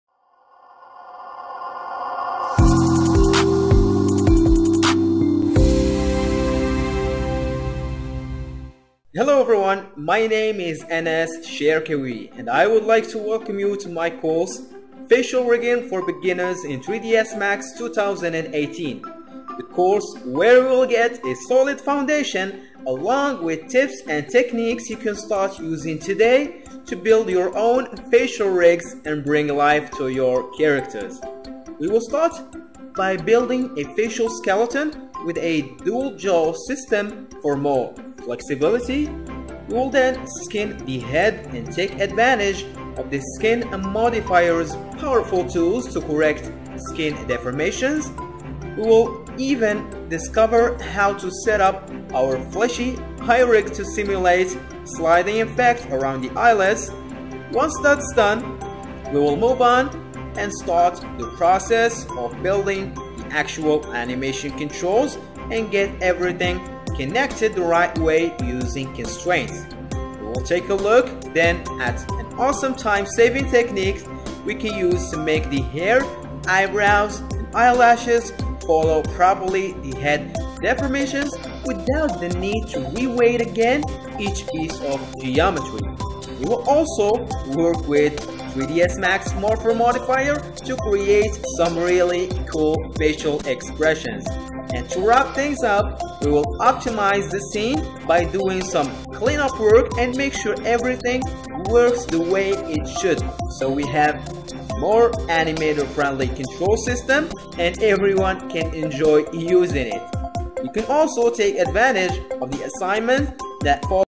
Facial Rigging For Beginners in 3ds Max 2018 - learn 3D Rigging
What you'll learnHow to build a facial skeleton with a dual jaw bones to get more flexibilty.How to skin your model and use the skin modifier's features to improve the deformations.How to build a fleshy eye rig to simulate sliding around the eyelids.How to build animation controls.How to connect animation controls with bone objects using constraints.How to use inverse kinematics as an alternative way to control the eyes.How to create custom parameters.How to drive the tongue using wire parameters.How to use the skin wrap modifier to attach the hair, eyebrows and eyelashes to the Head deformations.How to create awesome looking facial expressions.
Are you Interested in the world of 3D Animation and Facial Rigging but don't know how and where to start? Then this course is for you.
My name is Anass Cherkaoui and I will be very glad to assist you and walk you step by step into the most entertaining field in the world.
With every lesson, you will develop new skills and improve your understanding about 3D Rigging, especially Facial Rigging and discover the various tools and techniques you can use to rig your models more effectively.
This course is full of fun and excitement while at the same time delivering high quality content that's applicable to every other 3D software besides Autodesk 3ds Max. It's structured as follows:
Chapter 01: Creating Bone ObjectsChapter 02: Skinning ProcessChapter 03: Building Animation ControlsChapter 04: Assignment
Furthermore, the course comes with an assignment at the end, so not only you will build a solid foundation about 3D Facial Rigging, but you will also get your hands-on practice Rigging your own 3D Characters' faces.
So, What are you waiting for? I can't wait to start this amazing journey with you. Let me help you build the necessary skills to start creating your own Facial Rigs TODAY.
this is the best 3D Rigging course. you can download and watch for free after enroll.